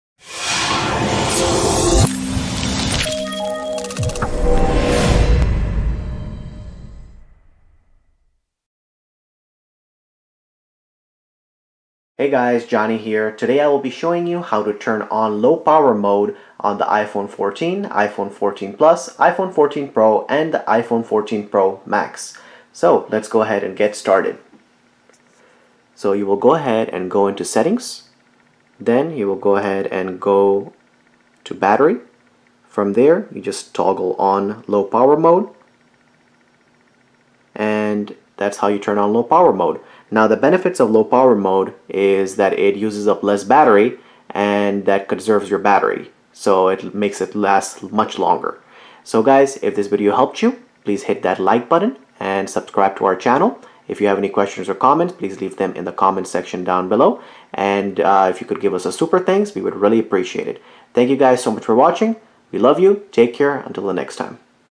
How to Turn ON Low Power Mode (Battery): iPhone 14/iPhone 14 Plus/iPhone 14 Pro/iPhone 14 Pro Max
- In this video I will show you how to turn on low power mode on the iPhone 14, iPhone 14 Plus, iPhone 14 Pro, and the iPhone 14 Pro Max.